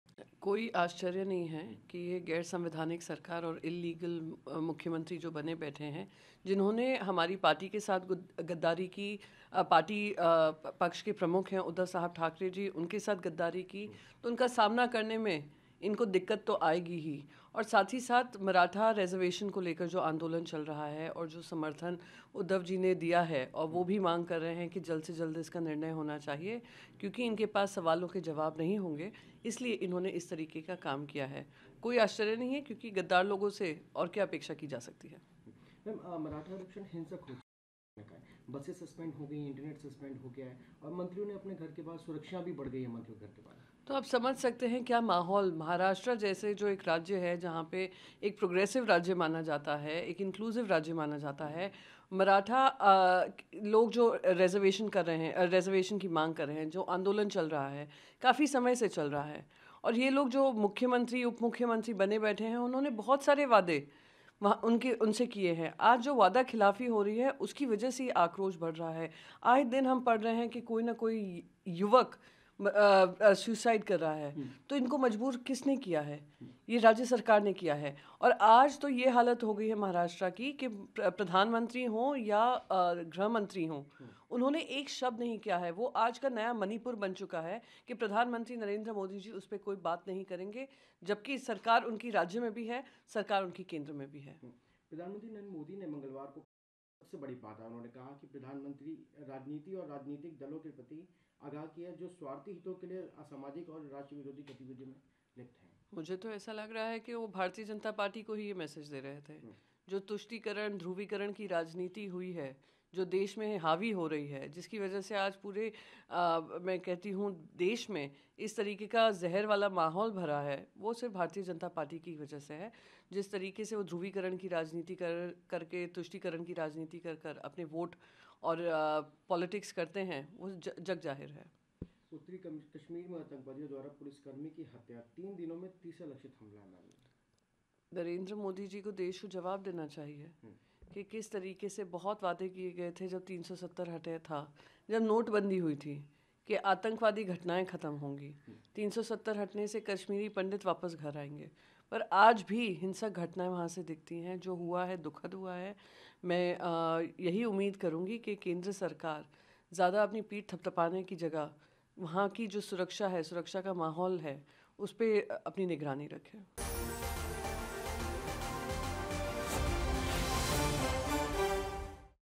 मराठा आंदोलन पर Shivsena ने राज्य सरकार को घेरा, PM Modi से भी पूछे सवाल | Chunav Aajtak
Maharashtra में भड़के मराठा आरक्षण आंदोलन को लेकर Shivsena (यूबीटी) ने राज्य सरकार पर निशाना साधा है। पार्टी की नेता प्रियंका चतुर्वेदी ने कहा है कि राज्य सरकार के धोखे के कारण आंदोलन जोर पकड़ रहा है। उन्होंने पीएम नरेंद्र मोदी से भी इस मुद्दे पर सवाल पूछे हैं।